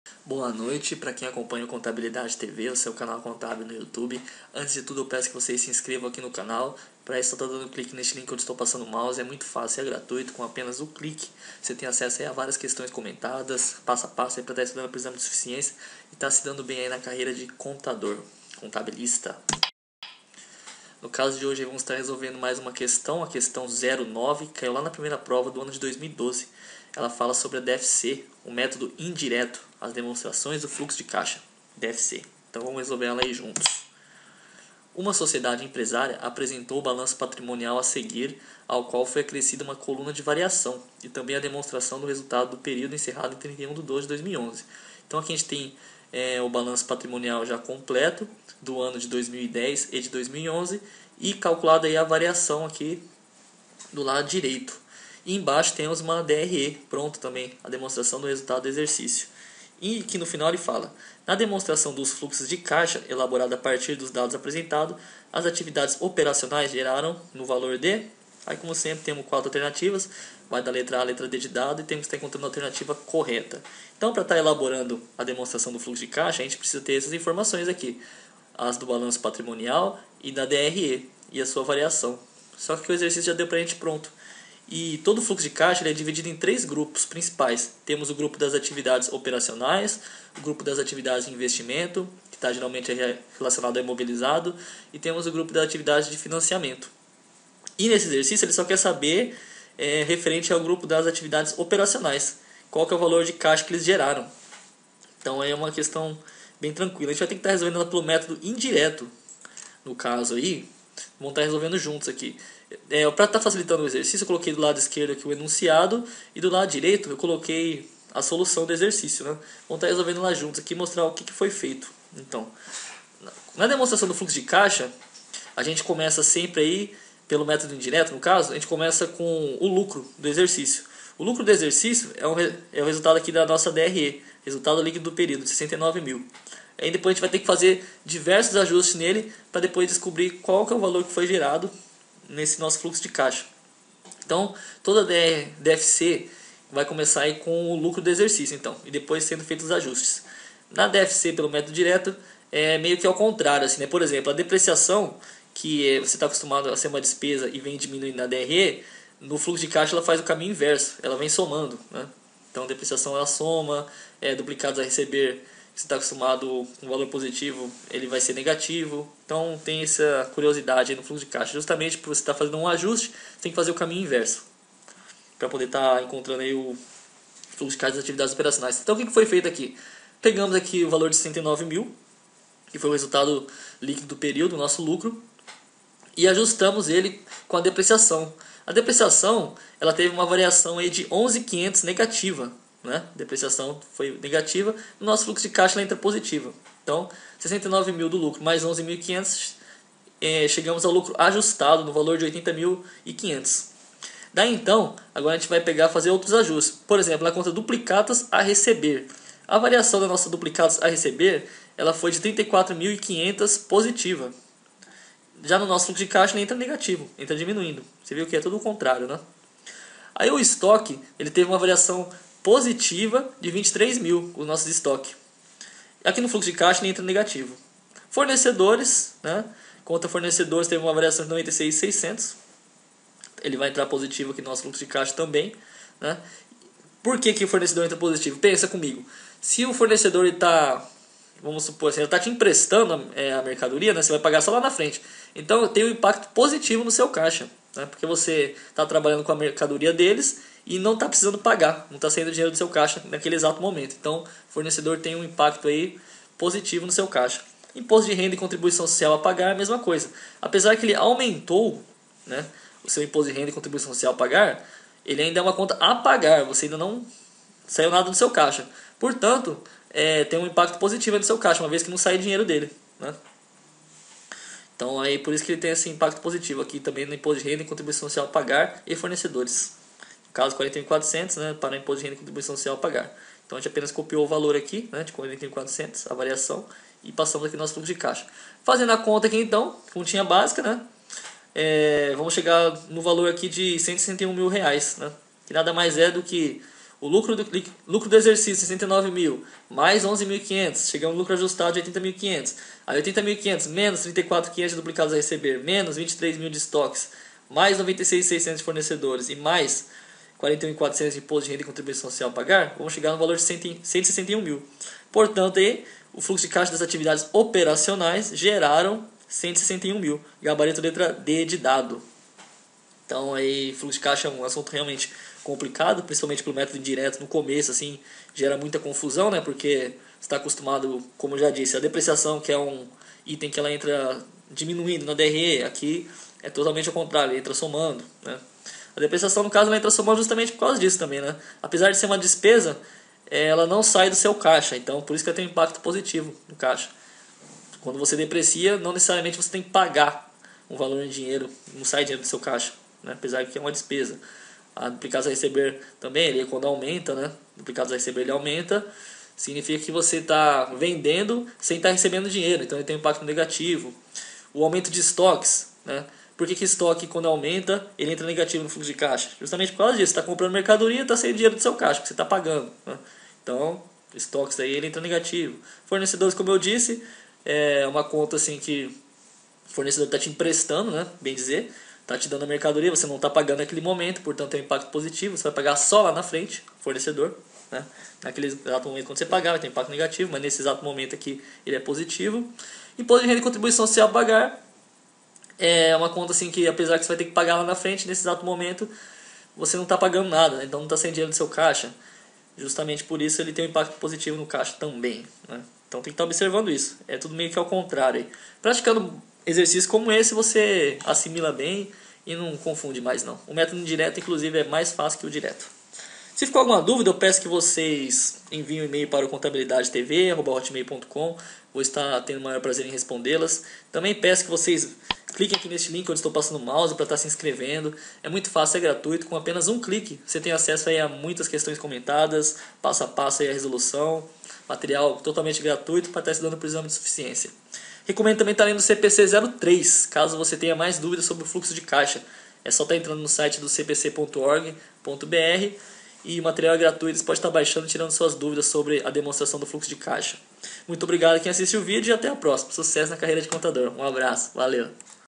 Questão 09 exame de suficiencia 2012 prova 1 - DFC
9. Uma sociedade empresária apresentou o Balanço Patrimonial a seguir, ao qual
foi acrescida uma coluna de variação, e também a Demonstração do Resultado
do período encerrado em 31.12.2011:
Balanço Patrimonial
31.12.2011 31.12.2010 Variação
ATIVO CIRCULANTE R$322.000,00 R$230.000,00 R$92.000,00
Caixa R$57.500,00 R$23.000,00 R$34.500,00
Duplicatas a Receber R$195.500,00 R$161.000,00 R$34.500,00
Estoques R$69.000,00 R$46.000,00 R$23.000,00
ATIVO NÃO CIRCULANTE R$115.000,00 -- R$115.000,00
Imobilizado R$126.500,00 -- R$126.500,00
(-) Depreciação Acumulada (R$11.500,00) -- (R$11.500,00)
TOTAL DO ATIVO R$437.000,00 R$230.000,00 R$207.000,00
PASSIVO CIRCULANTE R$184.000,00 R$46.000,00 R$138.000,00
Fornecedores R$142.600,00 R$46.000,00 R$96.600,00
Imposto de Renda e
Contribuição Social a Pagar R$41.400,00 -- R$41.400,00
PATRIMÔNIO LÍQUIDO R$253.000,00 R$184.000,00 R$69.000,00
Capital R$184.000,00 R$184.000,00 --
Reservas de Lucros R$69.000,00 -- R$69.000,00
TOTAL PASSIVO + PL R$437.000,00 R$230.000,00 R$207.000,00
Demonstração do Resultado
Vendas Líquidas R$391.000,00
Custo da Mercadoria Vendida (R$207.000,00)
Resultado Bruto R$184.000,00
Despesas com Vendas (R$4.600,00)
Despesas com Pessoal (R$57.500,00)
Despesas com Depreciação (R$ 11.500,00)
Resultado antes dos tributos sobre o Lucro R$ 110.400,00
Tributos sobre o Lucro (R$ 41.400,00)
Resultado Líquido do Período R$ 69.000,00

gabarito D